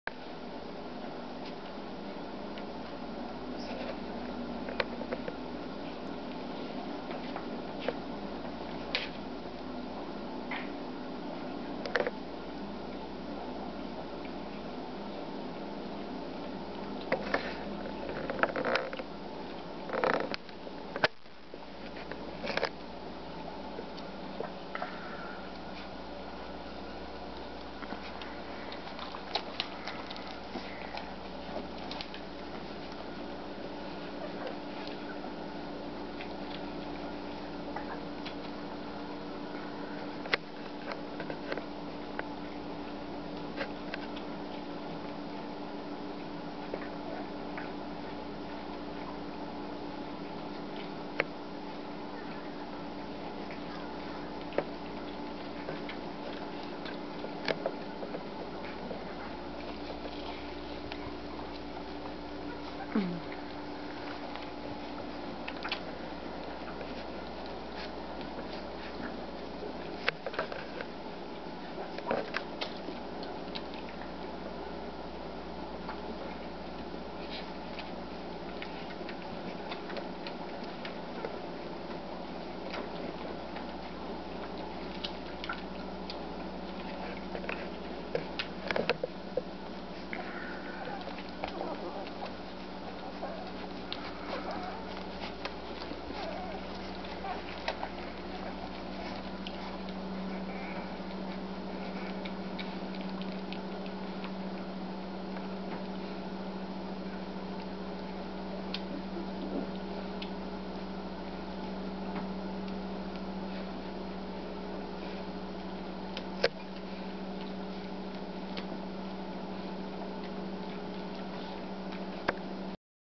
ISABELLA BABIES
isabella lola with her 1st litter of whites babies dobermans